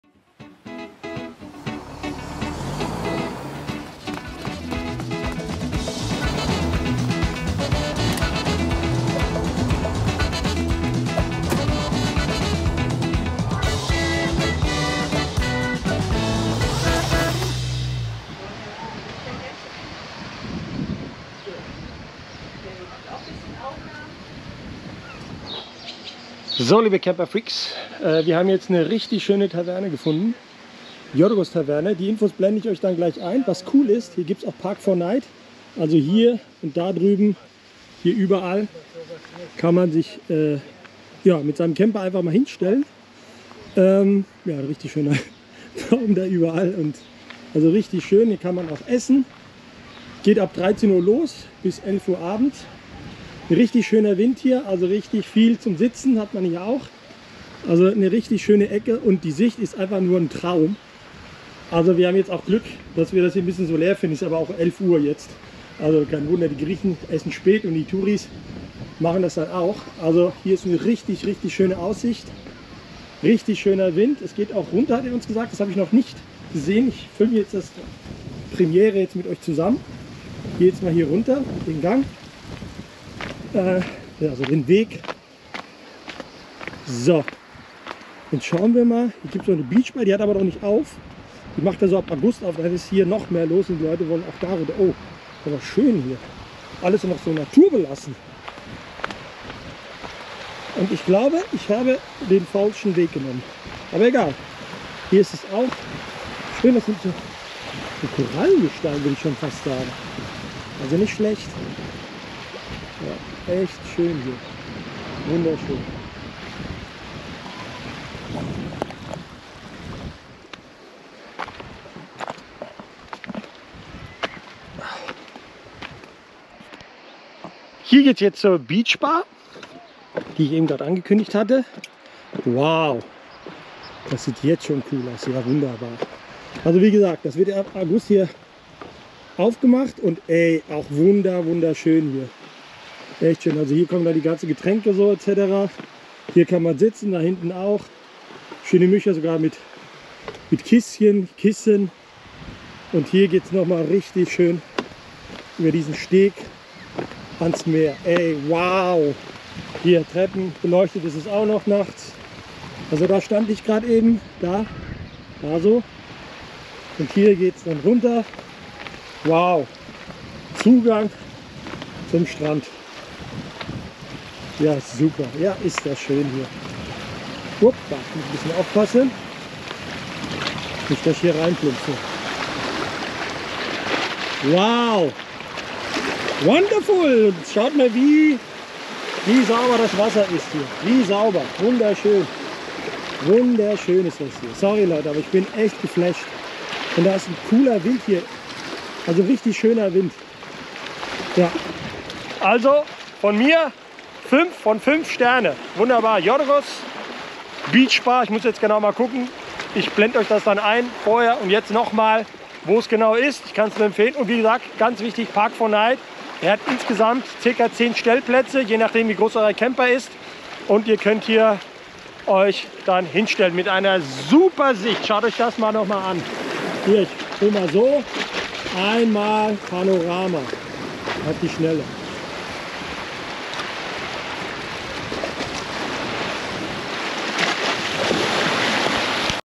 Taverna Faros in Peloponnes mit Stellplätzen für Camper und einer traumhaften Sicht aufs Meer
Tolle Taverne mit superschöner Aussicht und das Beste 10 Stellplätze direkt am Meer 😍

Adresse: Loutra Killinis in Arkoudi 27050 Griechenland - Peloponnes

DANKE!